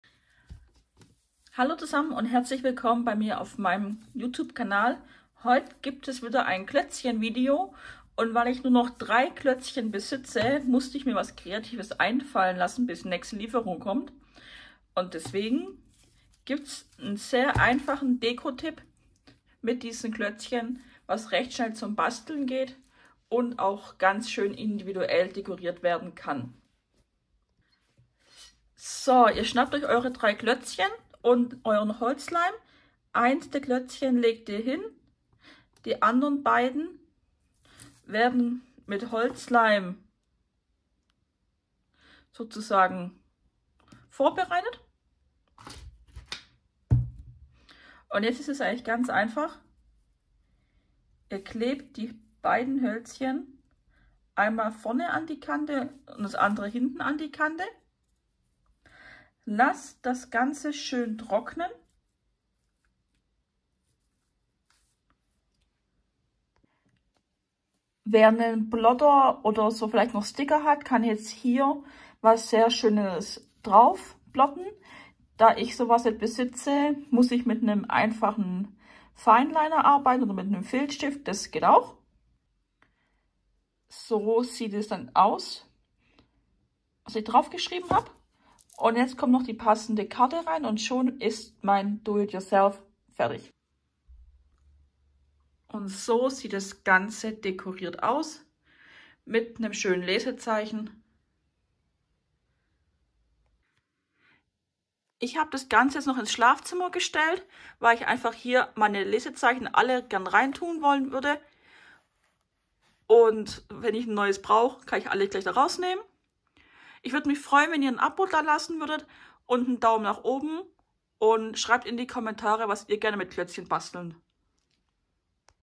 Postkartenhalter selber machen | Klötzchen DIY | Klötzchenliebe
Seid ihr im Klötzchenfieber? Dann willkommen auf meinem Deko Ideen Kanal.
In diesem Video zeige ich euch einen einfachen Postkartenhalter aus Klötzchen. Postkartenhalter selber machen bzw. Postkartenhalter selber bauen. Die Klötzchen Idee kam mir weil ich nur noch 3 Klötzchen hatte und aus den Holzresten etwas basteln wollte. Sozusagen eine Holzreste Idee. Ideal für ein kleines Deko Highlight. So lassen sich Postkarten schön präsentieren und aufbewahren. Ebenfalls eine schöne Geschenkidee. Wer Postkarten z.B auf Vistaprint erstellt kann mithilfe des Halters hier sehr schöne Geschenke selber basteln.

Ich veröffentliche regelmäßig Deko Ideen und Basteln Ideen für Klötzchen.